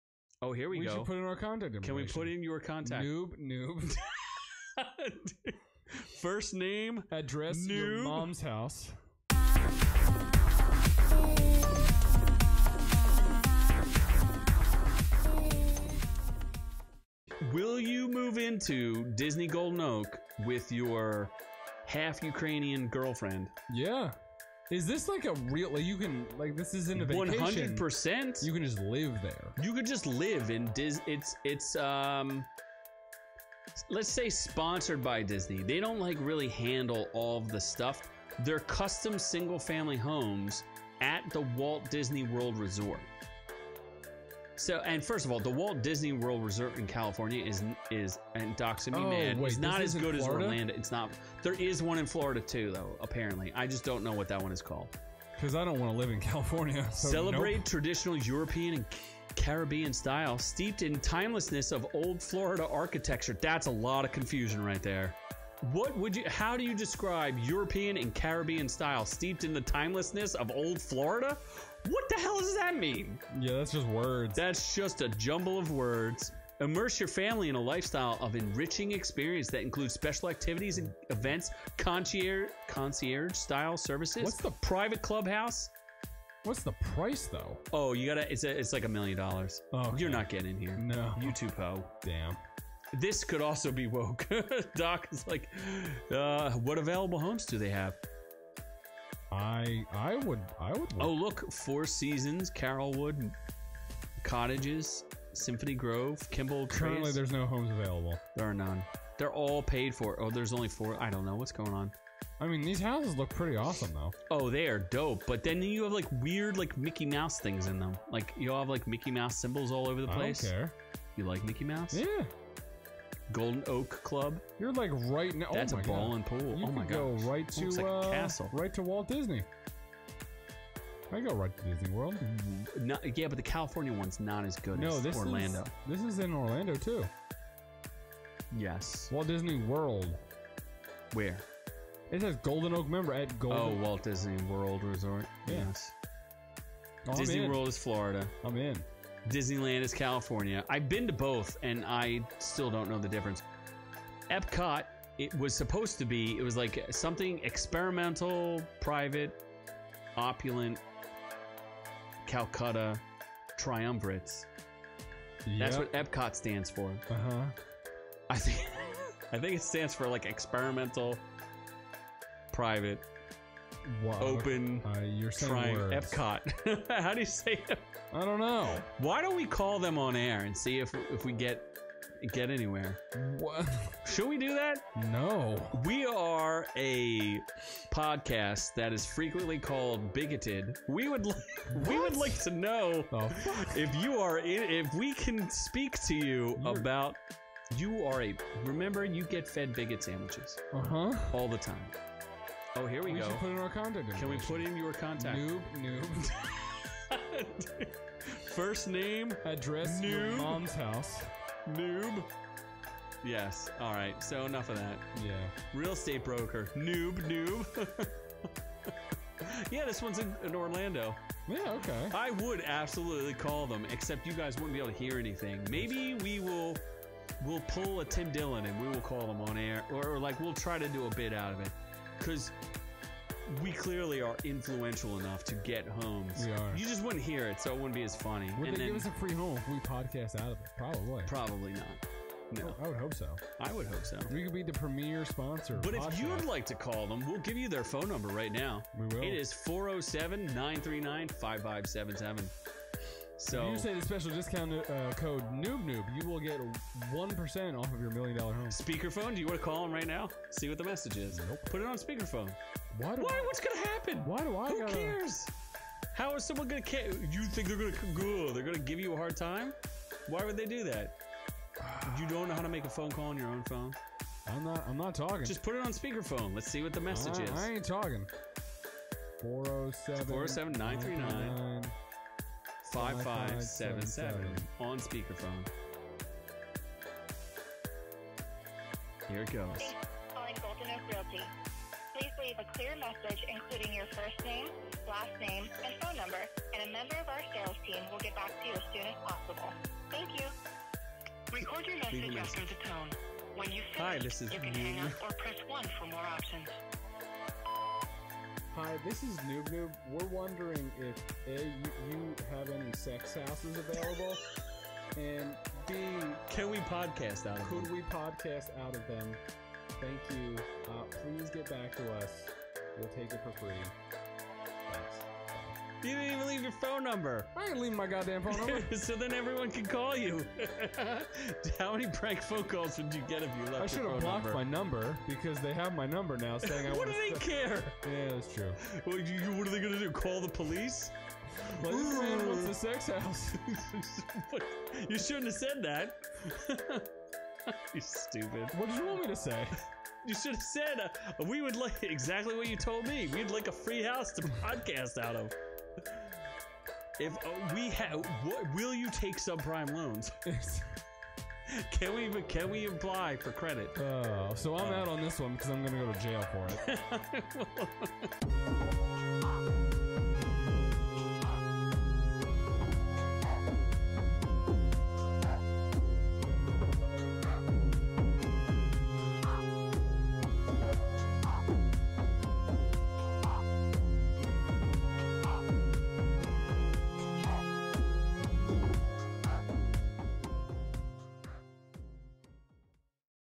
Disney Planned Communities Get Call From Our Podcast- ORKU Episode 234 Clip
Noob Noob and Z explore the possibilities in living with the Disney Corporation.